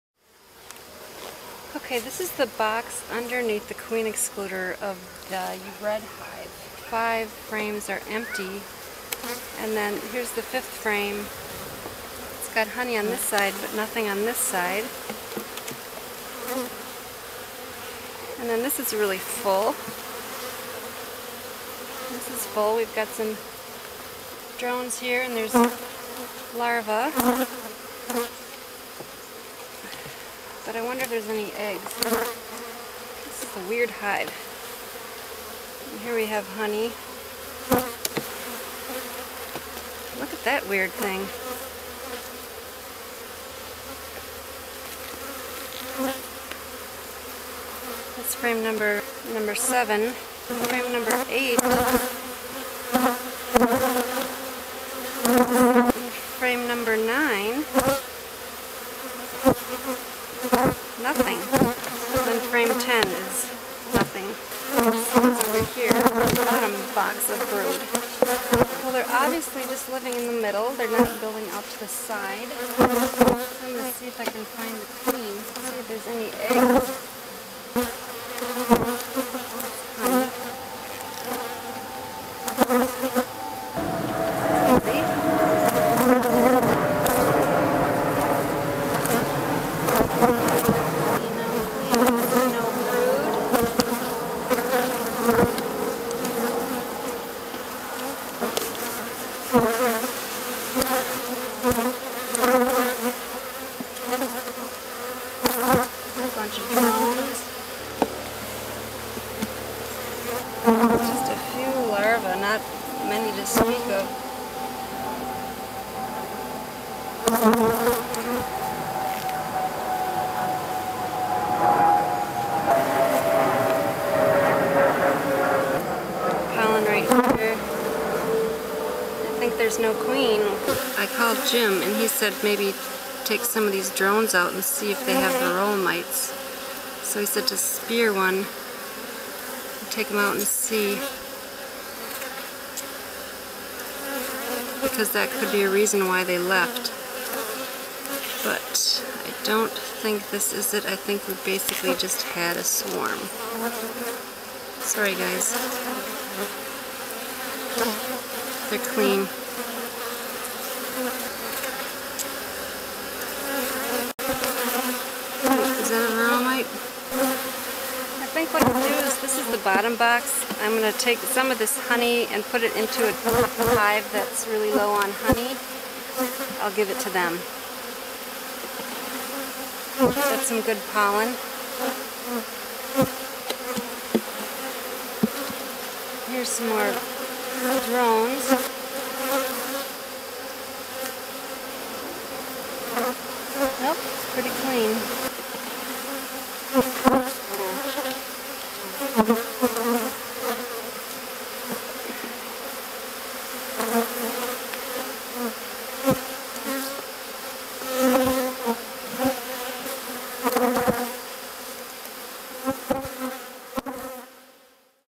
T McD's Bees bees - swarmed after honey harvest
Aug. 22nd I was checking the hives and noticed that there was no honey above the queen excluder. As I was checking the brood boxes I noticed that there were only drone cells left so it was suggested to take some of the drones out to see if they were infested with varroa mites; they were not so I would conclude that shortly after I harvested the honey on Aug. 5th the hive swarmed. Now this hive is too small to live through the MN winter, they would starve, so I salvaged some of the honey frames and put them into other hives. For an experiment I put all of the bees into one brood box and took a frame with brood and eggs from a strong hive and put it in the center of that box to see if they would make another queen.   I spelled Varroa mite wrong in the video sorry about that but I don't have time to change it now.